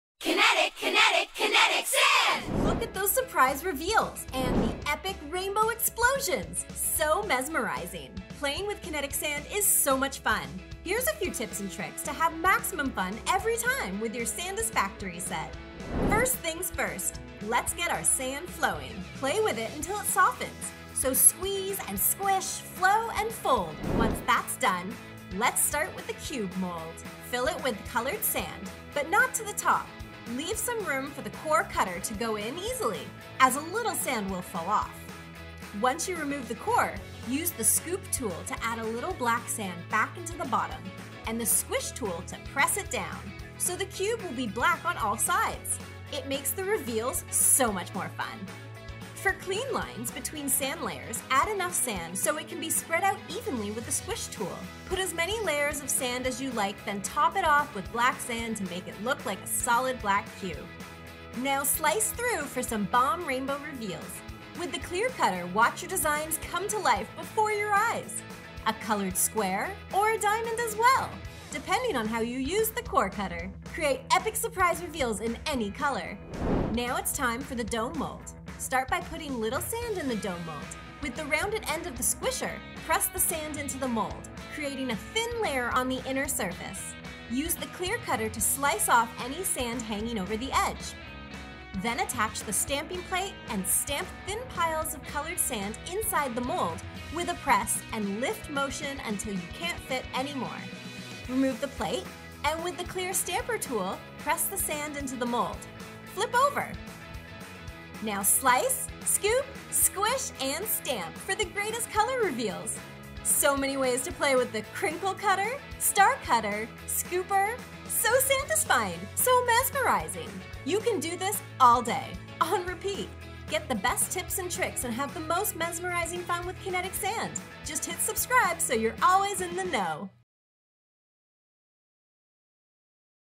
Kinetic Sand SANDisfying Set
Enter the mesmerizing world of Kinetic Sand with this SANDisfying set. 🤩

Scoop, squish and slice your creativity with 10 different tools to mold your desired shape. Here are some TIPS on how to create an amazing masterpiece with our colorful sands! 🌈🖐🔪

Now available at all leading toy stores nationwide! 🛒